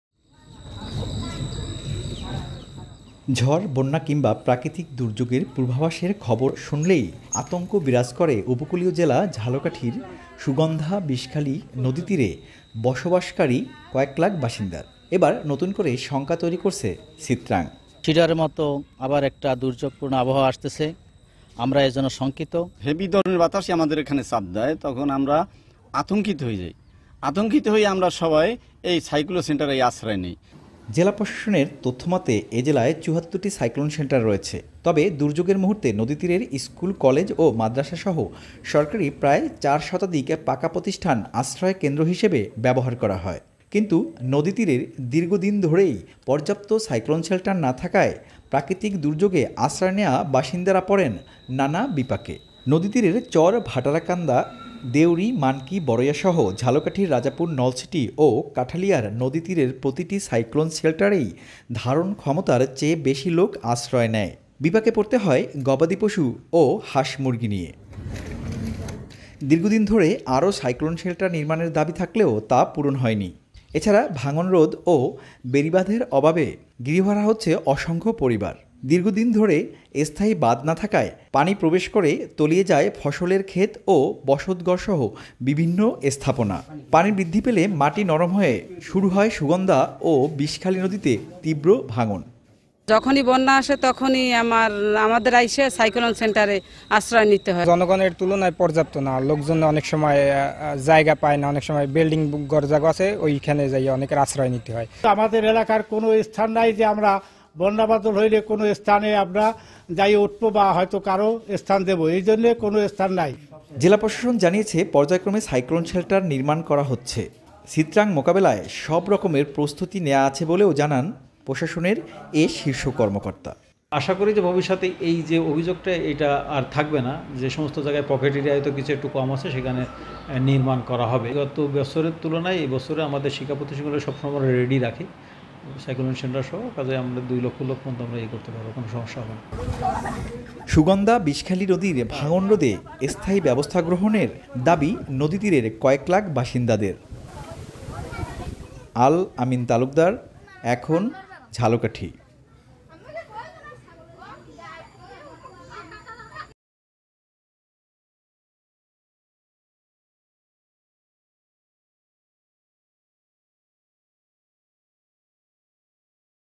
দুর্যোগ মোকাবেলায় কতটা প্রস্তুত ঝালকাঠির নদীপাড়ের মানুষ? | Jhalakathi Shelter | Ekhon TV
উপকূলীয় জেলা ঝালকাঠির সুগন্ধা-বিষখালী নদী পাড়ের লাখো মানুষের জন্য নেই পর্যাপ্ত সাইক্লোন শেল্টার। ফলে সিত্রাং আতঙ্কে নদীপাড়ের গ্রামীণ জনপদের মানুষ। জেলা প্রশাসন বলছে, ৭৪টি সাইক্লোন শেল্টারসহ ৪২৭টি স্কুল, কলেজ ও মাদ্রাসা প্রস্তুত রাখা হয়েছে, তাতে ৪ লাখ মানুষকে আশ্রয় দেয়ার ব্যবস্থা করা যাবে।

Our Office Address:
EKHON TV
City Park Lane
19 Hatkhola Road, Wari
Dhaka-1203
Bangladesh